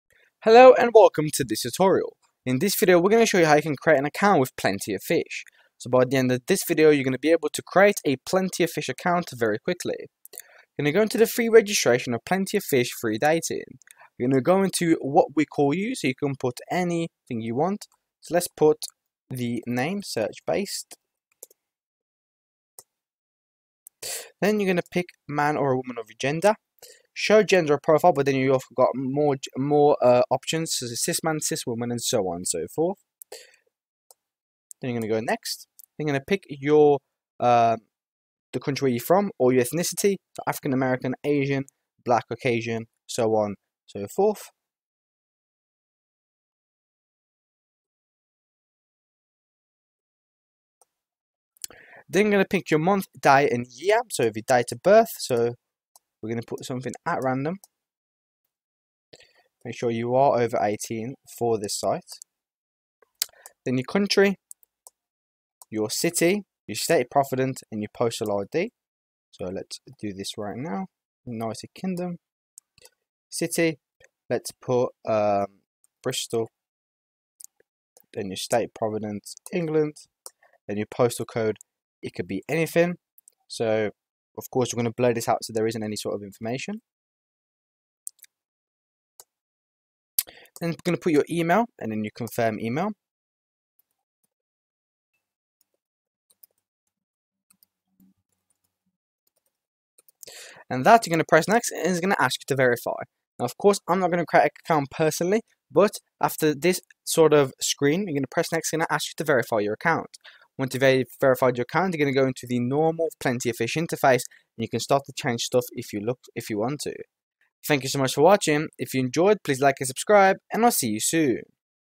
How To Create Plenty of Fish POF Account | Tutorial
How To Create Plenty of Fish POF Account

Plenty of Fish (POF) is a Canadian online dating service, popular primarily in Canada, the United Kingdom, the Republic of Ireland, Australia, New Zealand, Spain, Brazil, and the United States. It is available in nine languages. The company, which is based in Vancouver, British Columbia generates revenue through advertising and premium memberships. While it is free to use, Plenty of Fish offers premium services as part of their upgraded membership, such as allowing users to see who has "liked" a member through the service's MeetMe feature, and whether a message has been read and/or deleted.